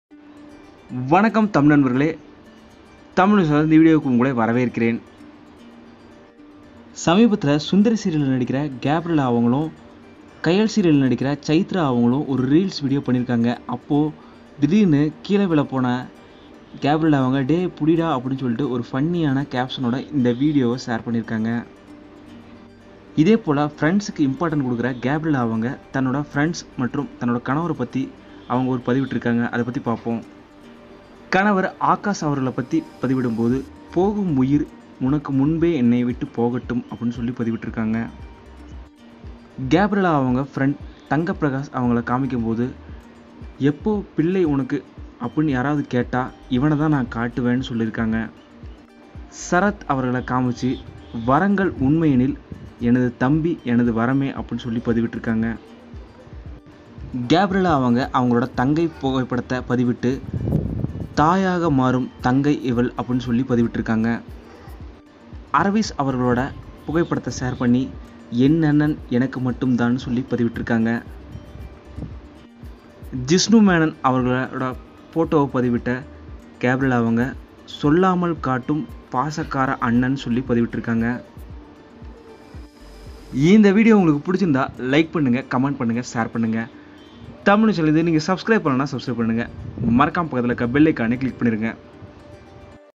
Dance ஆடும் போது தவறி விழுந்த சுந்தரி சீரியல் நடிகை | Gabrella dance fun video
Gabrella dance fun video.